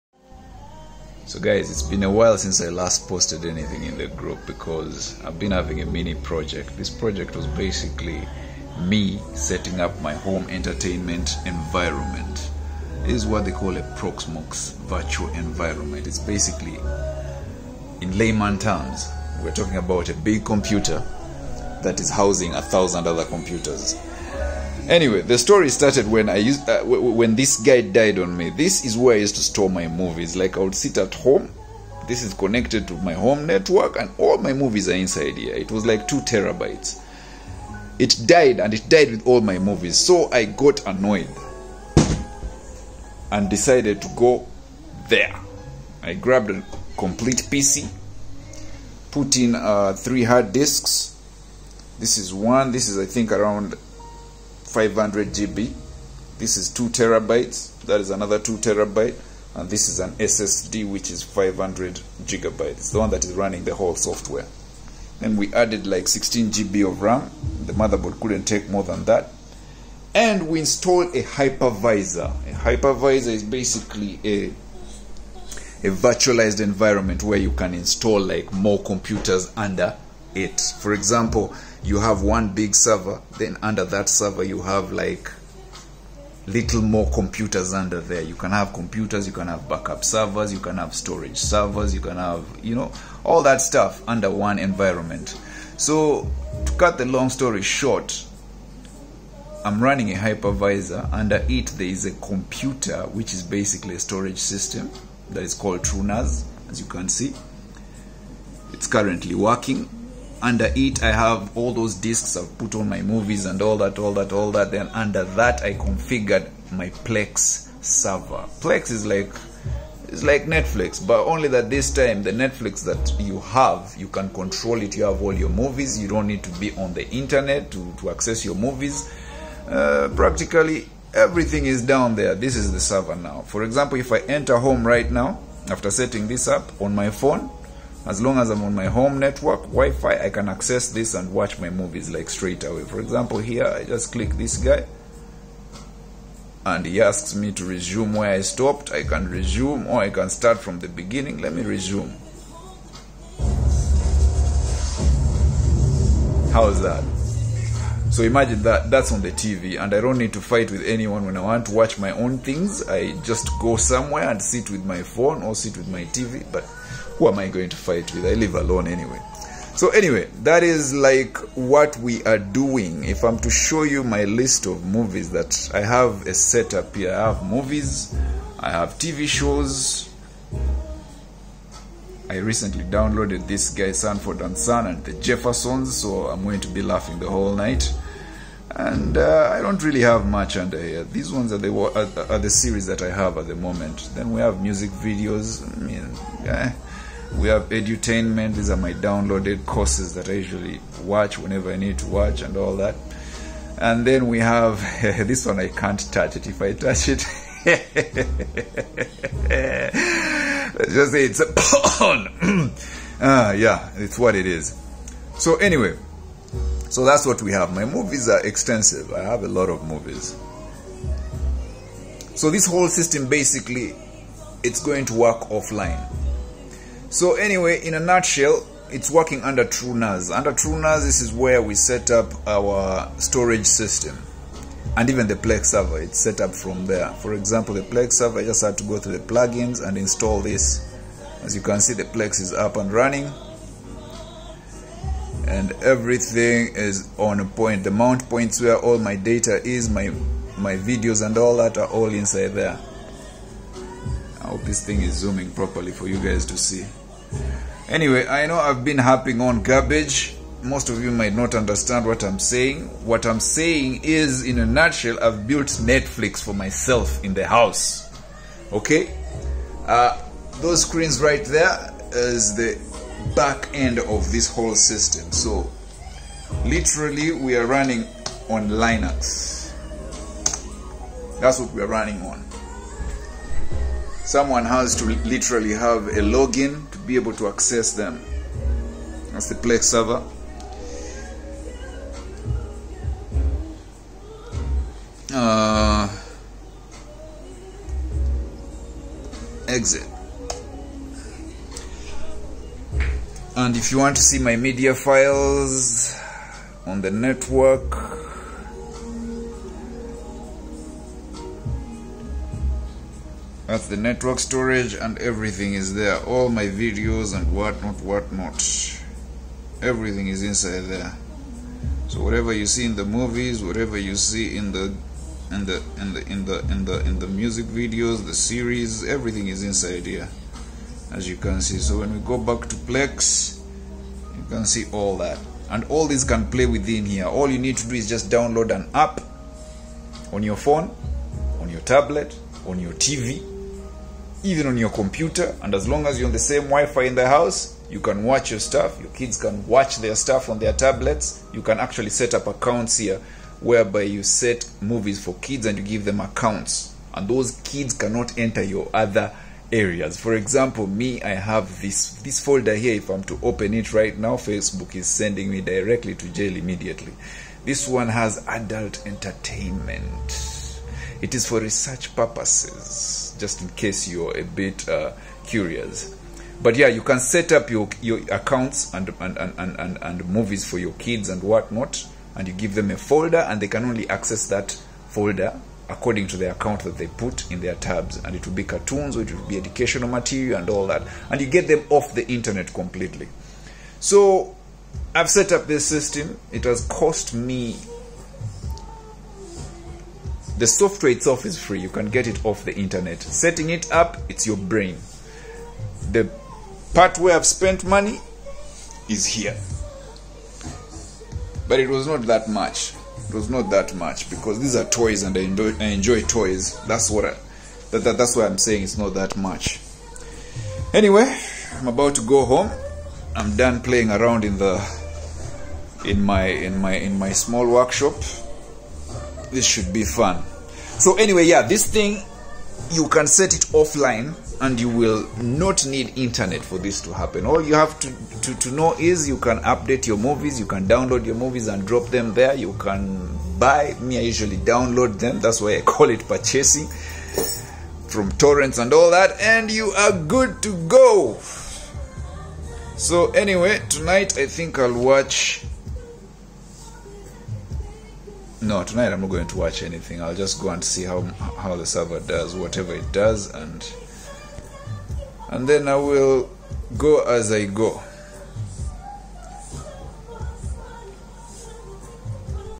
My Plex home server build running under Truenas in a Proxmox virtual environment 🤣
Proxmox virtual environment with Truenas running as a virtual machine housing Plex media server